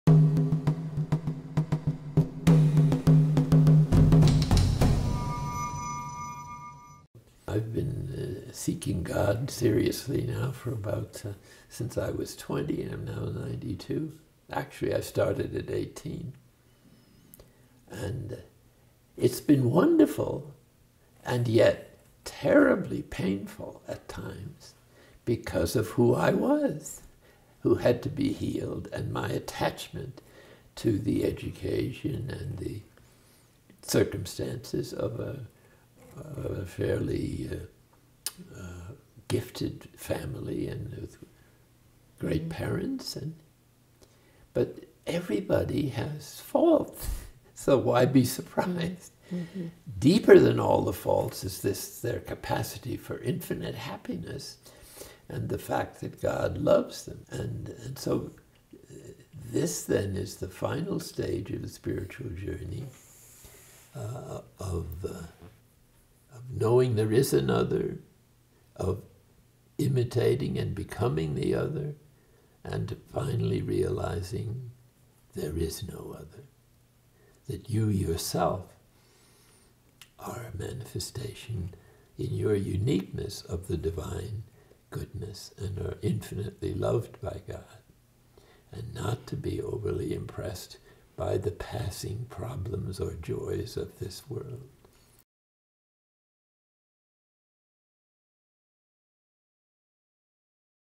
WISE WORDS: THOMAS KEATING my life
A wise spiritual leader has died and his life is being celebrated today, at this very hour in St. Joseph's Abbey Spencer where a funeral mass is held. I am very thankful for having been able to interview him in 2015 and work together with him on his last book 'World Without End'. He guided me through his ideas that have made a difference to so many people around the world. Dear teacher, a sincere thank you. We'll miss you.